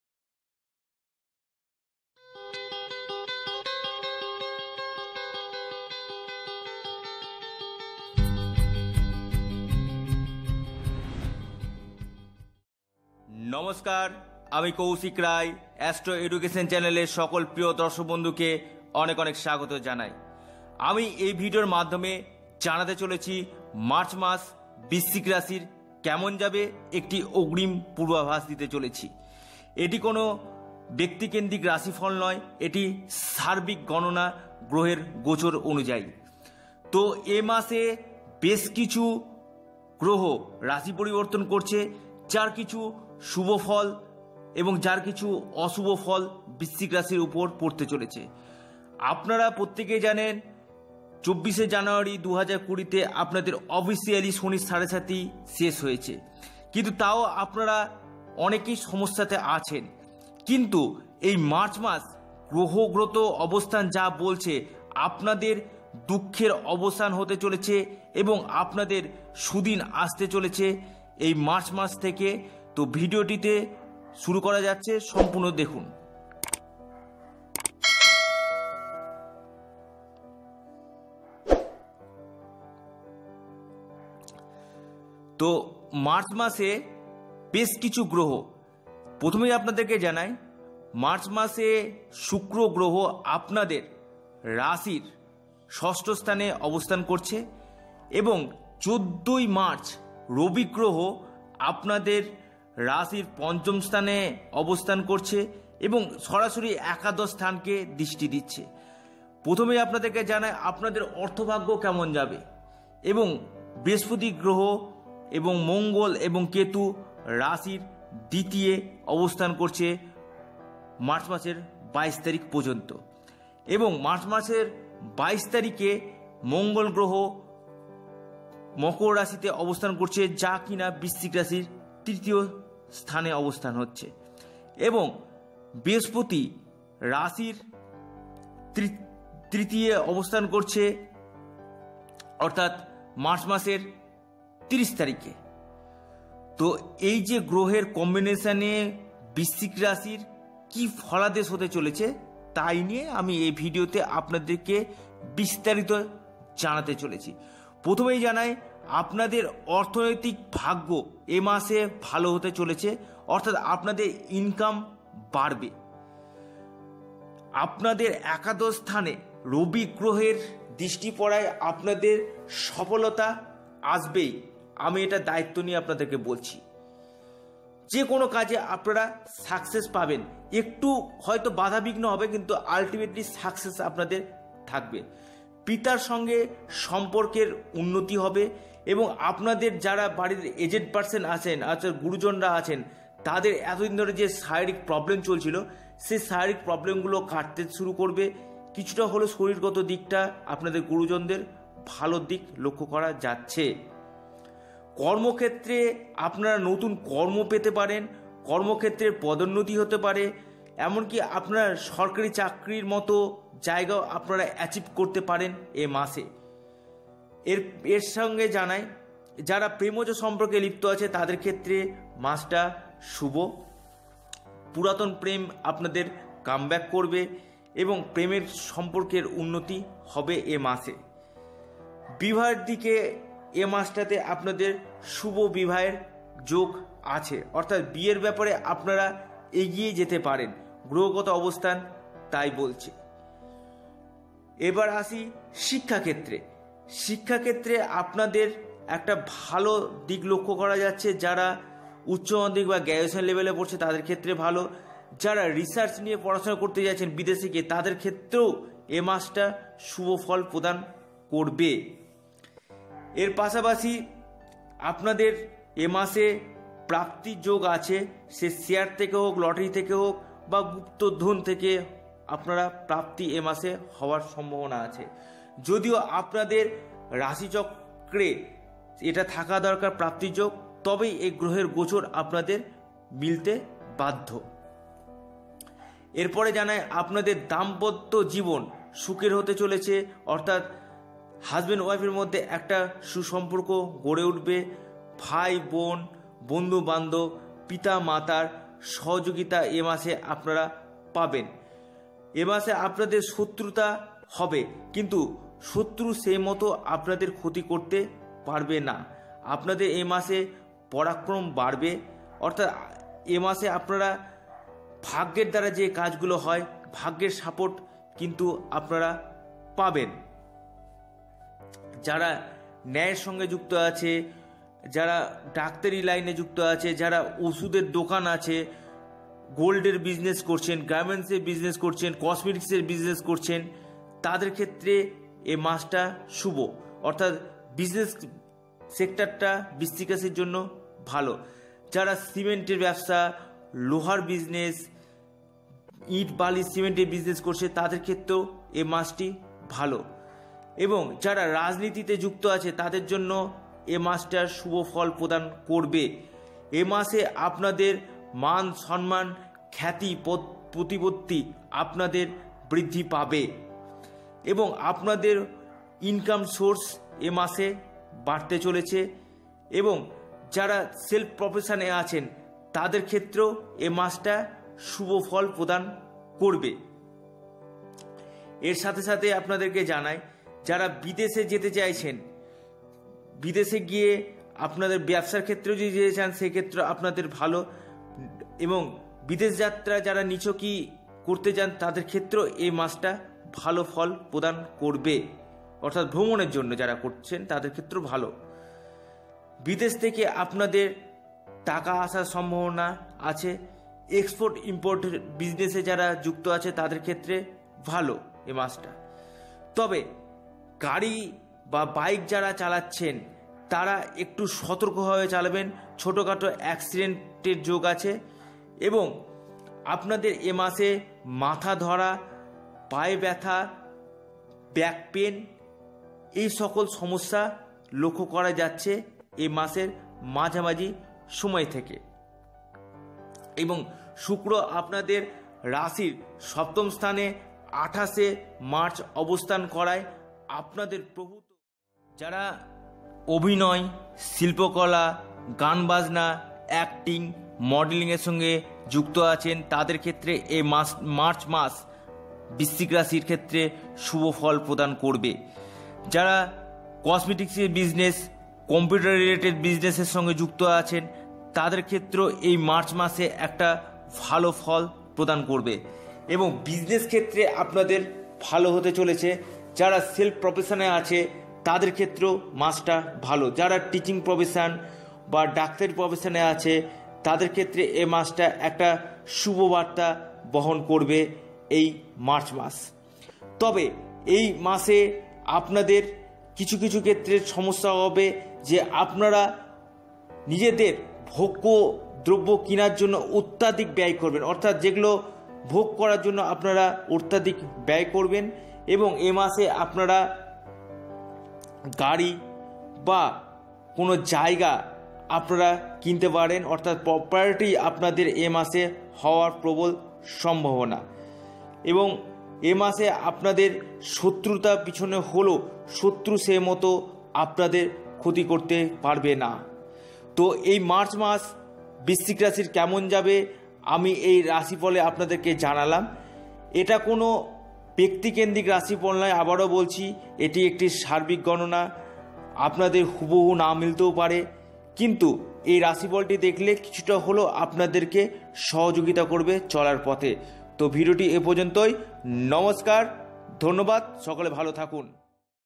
বৃশ্চিক রাশির মার্চ মাসের রাশিফল! Vrishchik Rashi March 2020 rashifal!
বৃশ্চিক রাশির মার্চ মাসে ধীরে ধীরে ভাগ্যের উন্নতি হবে অর্থ ও কর্ম ব্যবসা দাম্পত্য জীবন প্রেম বিবাহ কেমন যাবে এই ভিডিওতে অগ্রিম পূর্বাভাস দিতে চলেছে ধন্যবাদ!